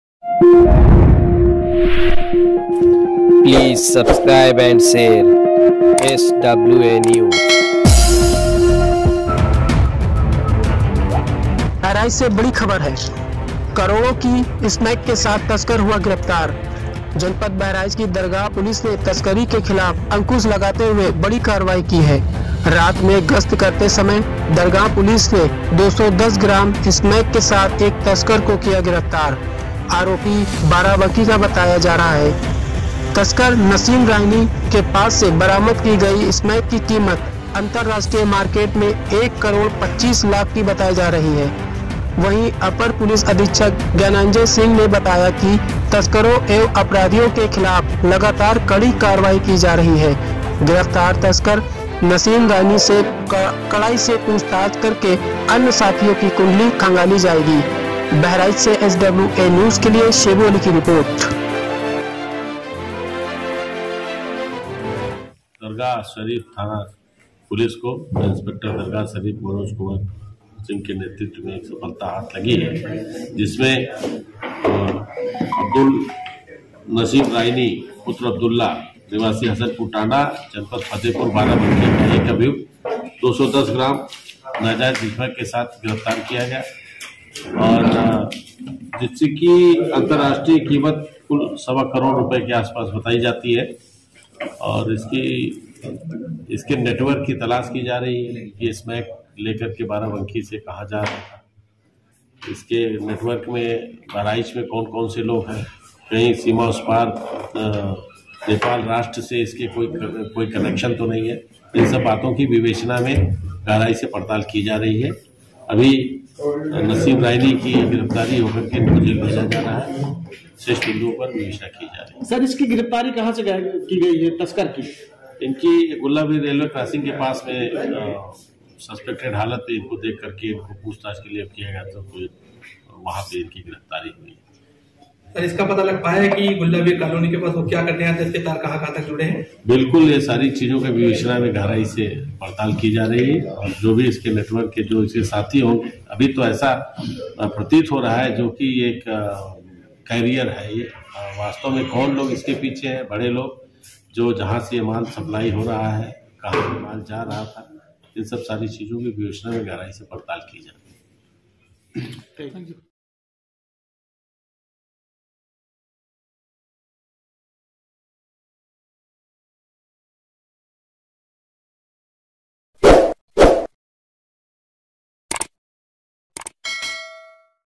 बहराइच - करोड़ो की स्मैक के साथ तस्कर गिरफ्तार
जनपद बहराइच की दरगाह पुलिस ने तस्करी के खिलाफ अंकुश लगाते हुए बड़ी कार्रवाई की है/
रात मे गस्त करते समय दरगाह पुलिस ने 210 ग्राम स्मैक के साथ एक तस्कर को गिरफ्तार किया है
आरोपी बाराबंकी का बताया जा रहा है तस्कर नसीम के पास से बरामद की गई स्मैक की कीमत अंतरराष्ट्रीय मार्केट में एक करोड़ पचचीस लाख बताई जा रही है
अपर पुलिस अधीक्षक ज्ञानेन्जय सिंह ने बताया कि तस्करों एवं अपराधियों के खिलाफ लगातार कड़ी कार्रवाई की जा रही है गिरफ्तार तस्कर नसीम रायनी से कड़ाई से पूछताछ करके अन्य साथियों की कुंडली खंगाली जाएगी/

 ज्ञानेन्जय सिंह ( अपर पुलिस अधीक्षक)

SWANEWS
/ ? start = hi Follow us on Google News for Breaking and Latest News Updates : SWANEWS India ( Hindi News ) :